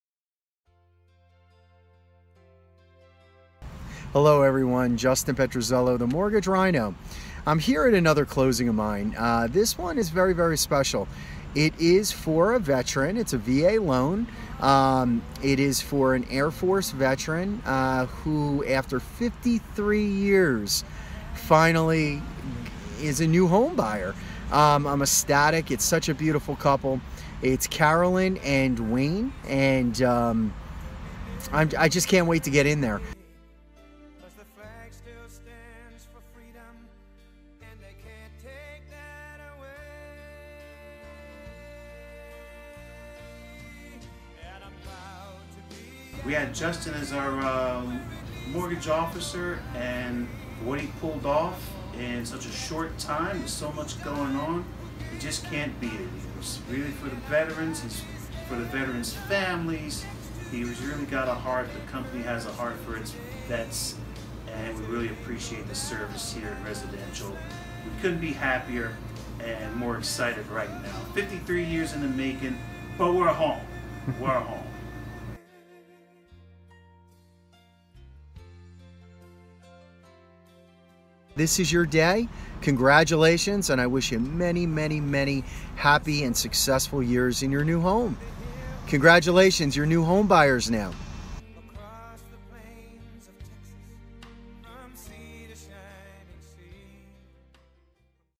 What A Proud Veteran
I am so proud to be a part of this day for Wayne & Caroline Jones. Wayne is an Air Force veteran who served during the Panama Crisis and was stationed in Panama in the early 1990s. Thank you for your service! Today is a great day for him and his family he fulfilled his dream of homeownership.  As you can see in the video it took him 53 years but he did it! During this process I have made true friends with Wayne and Caroline. They are amazing people! This dream home has aan enclosed porch in the back that I know Caroline loved it was easy access and made it easier with